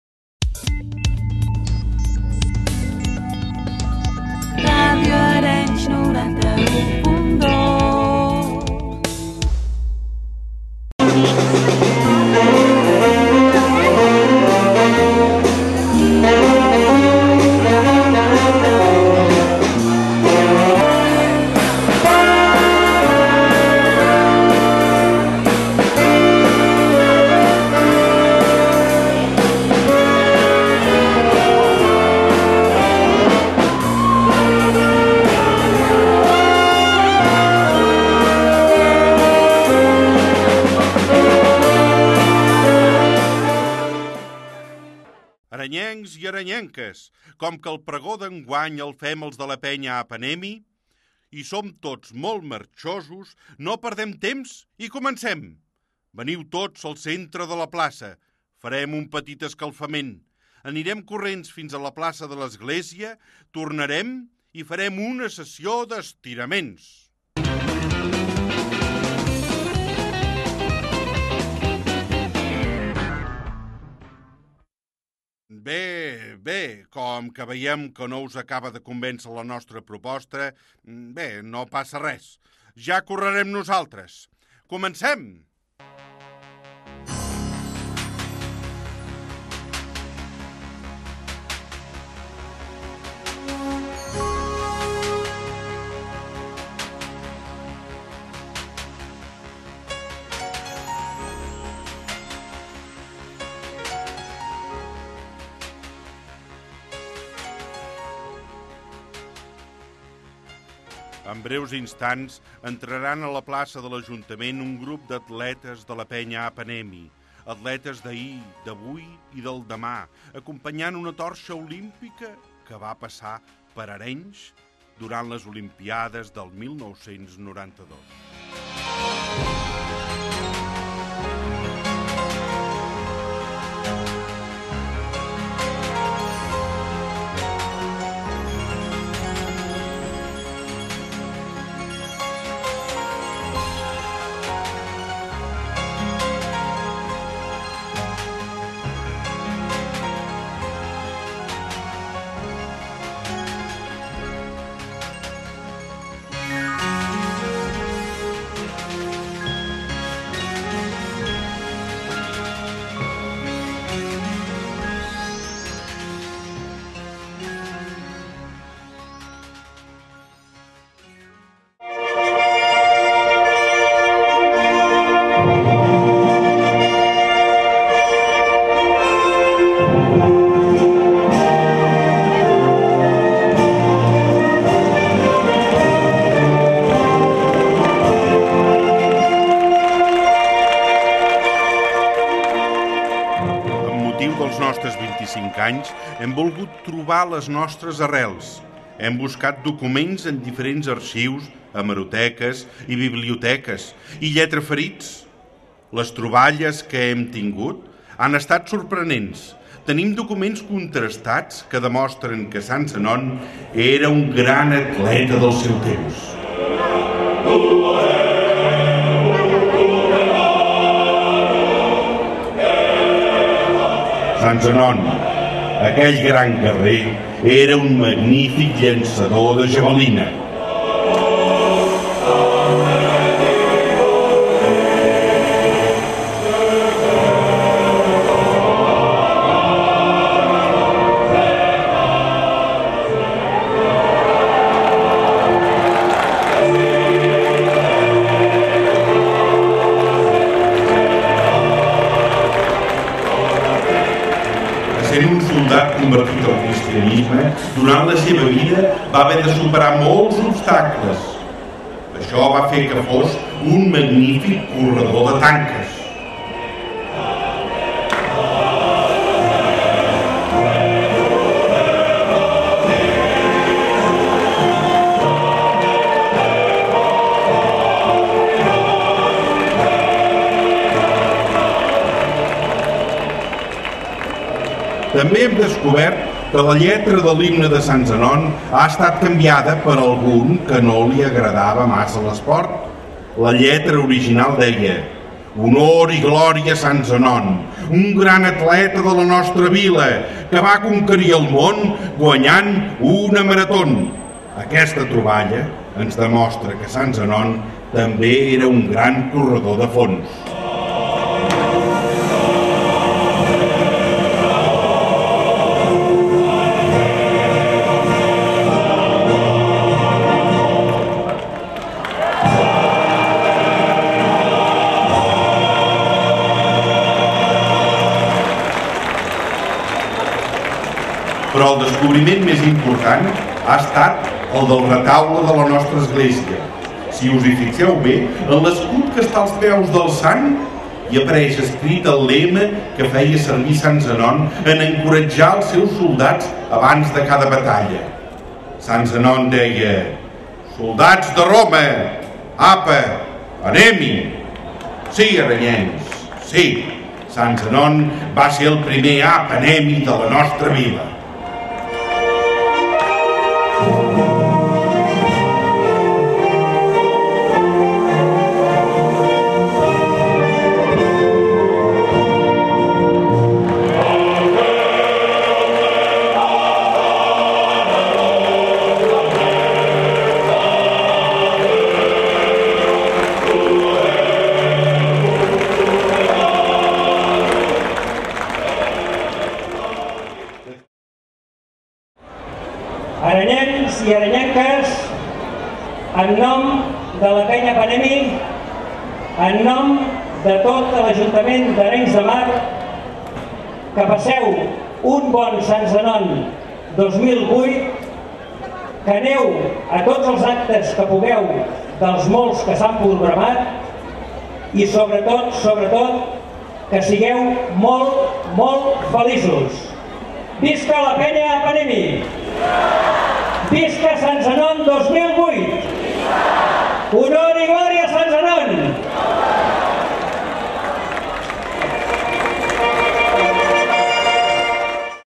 Pregó de Sant Zenon
Amb motiu del seu 25è aniversari, la Penya Apa Anem-hi va ser l'encarregada d'obrir la festa major d'aquest any amb un pregó original en el qual van partipar molts dels atletes de la penya.